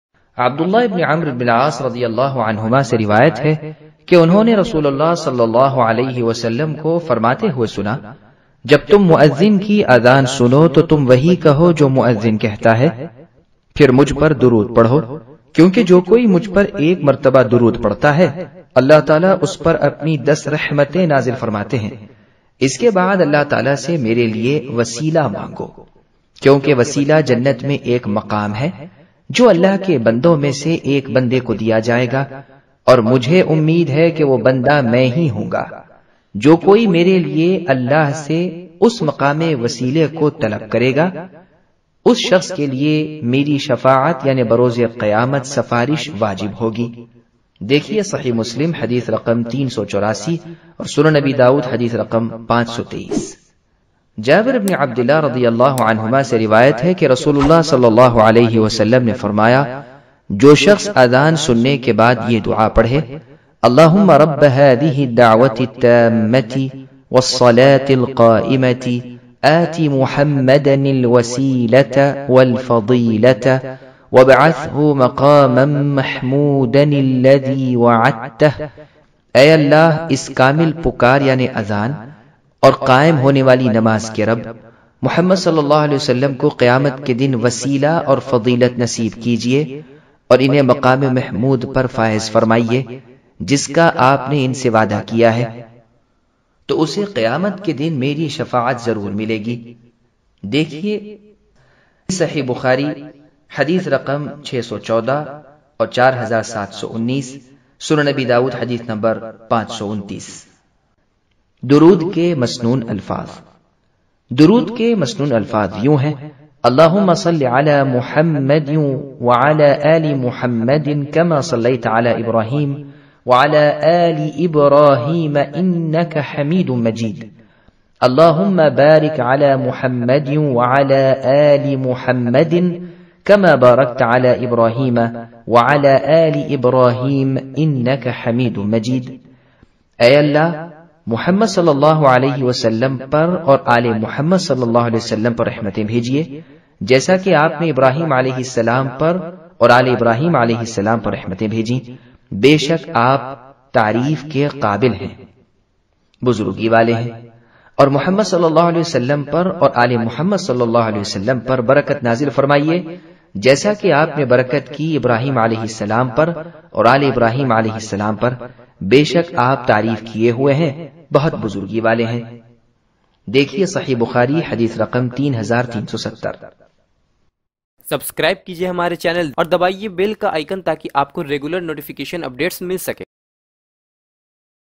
Muhammadﷺ ki Shifaat chahiye tu yeh kaam karo || IslamSearch
Azan ke bad ki Dua aur Darood Shareef ki Fazilat
Post Productions by Alauddeen Productions

This is IslamSearch official youtube channel